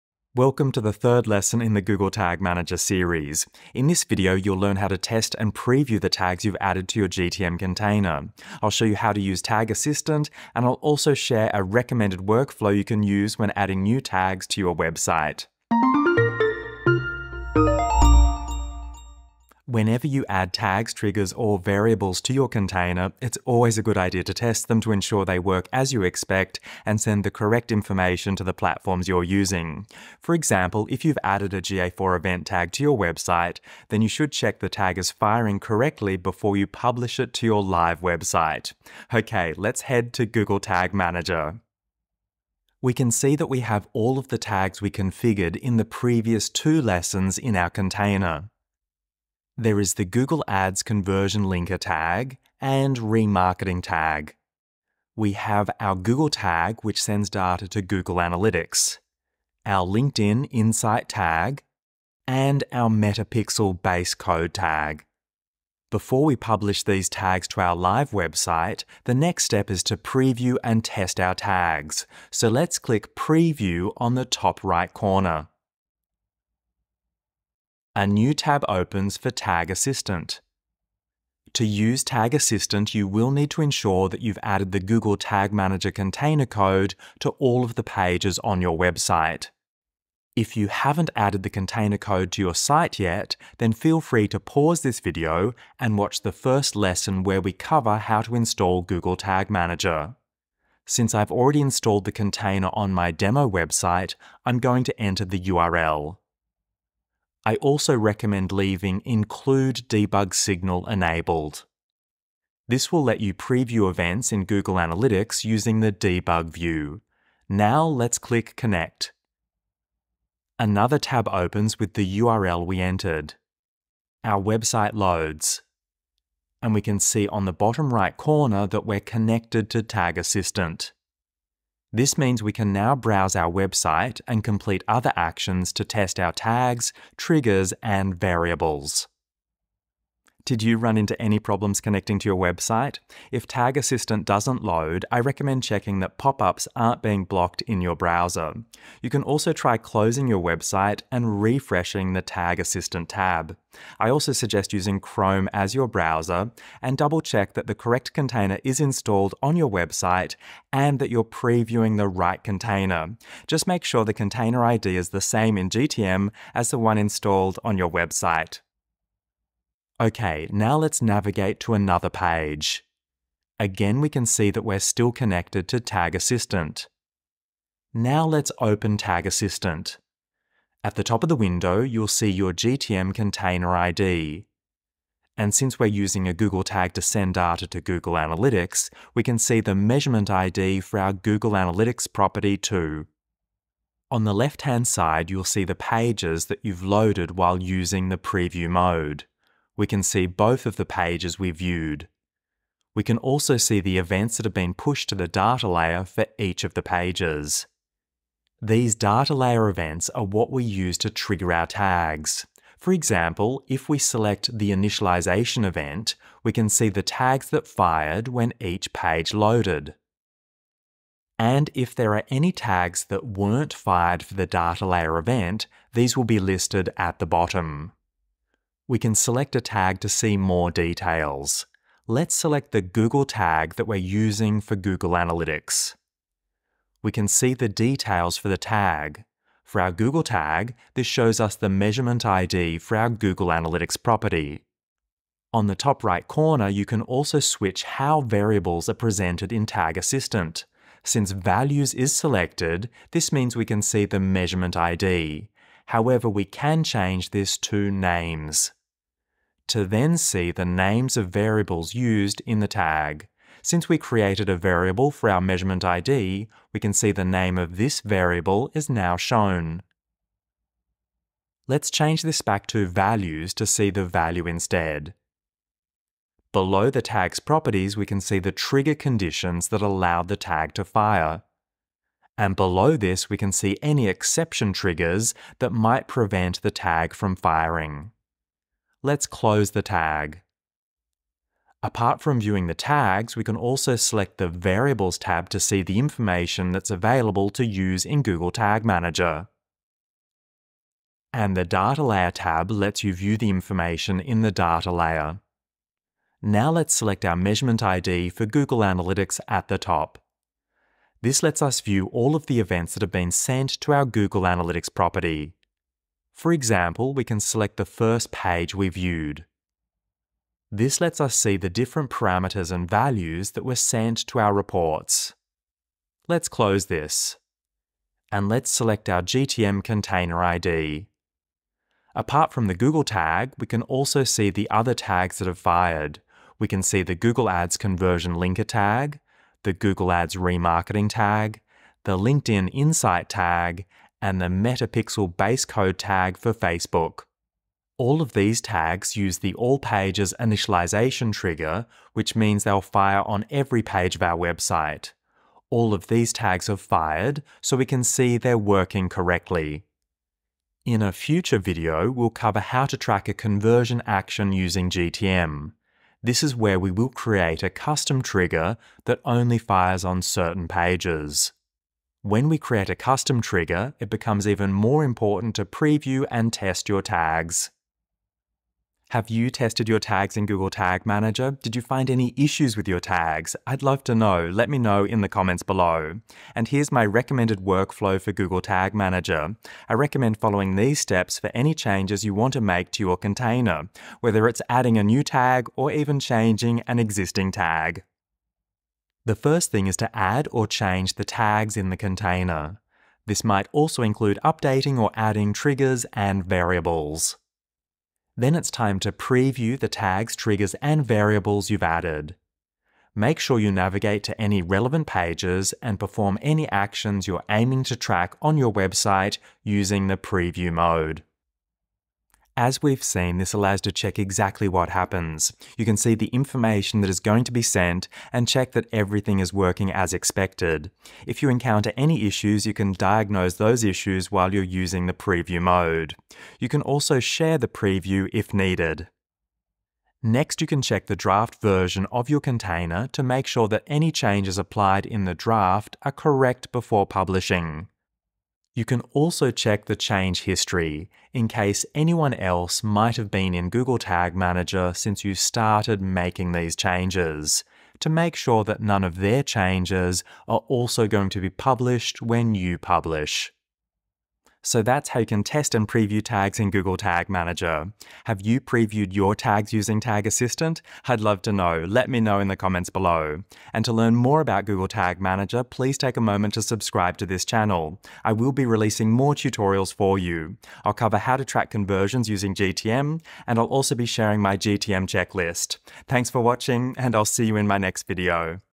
How to Preview and Test Tags in Google Tag Manager – GTM Tutorial Lesson 3
Learn how to preview and test your tags in Google Tag Manager using Tag Assistant. This is lesson three of our Google Tag Manager training series, where we will use the preview mode to check that our tags are firing correctly and to view the information that can be used by Google Tag Manager.

JUMP TO A TOPIC:
00:00 Introduction
00:41 Enabling preview mode
02:48 Previewing tags, triggers, and variables
06:26 Recommended workflow
07:44 Conclusion